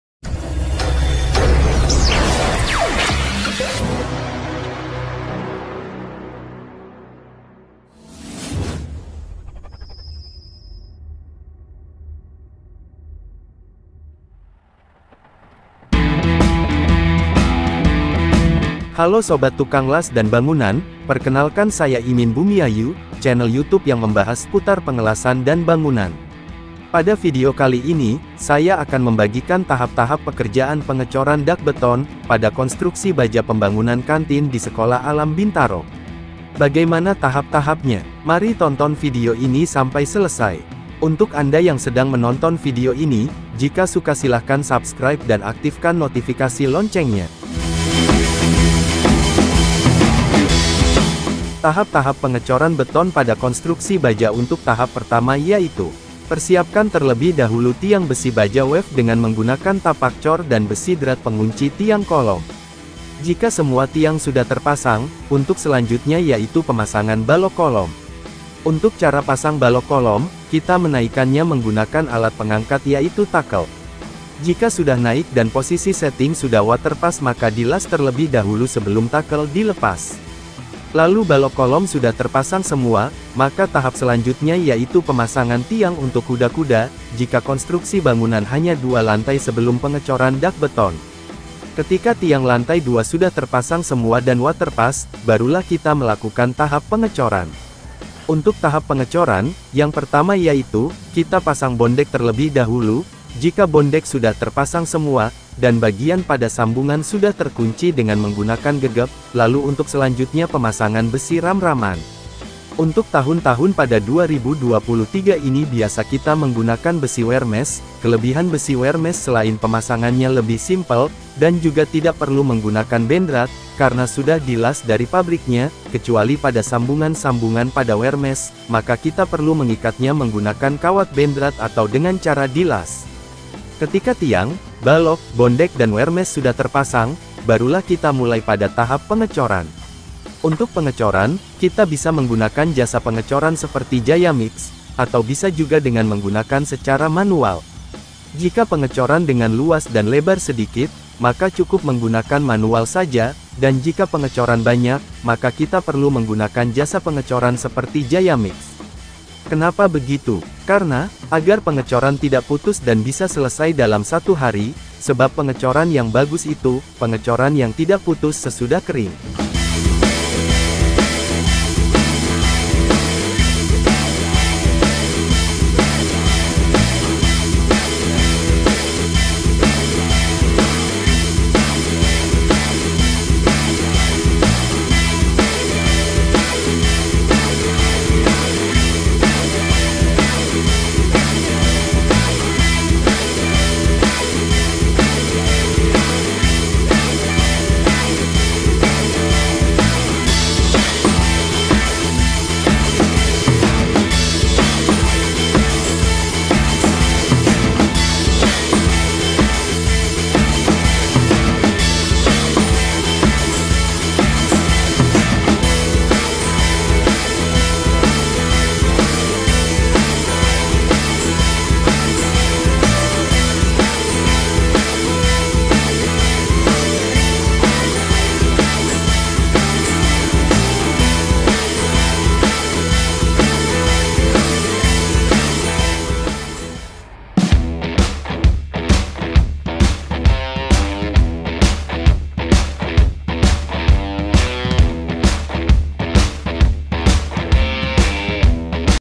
Membuat Konstruksi Baja WF Rumah 2 Lantai Sampai Ngedak
Pembangunan Kantin Di Sekolah alam bintaro.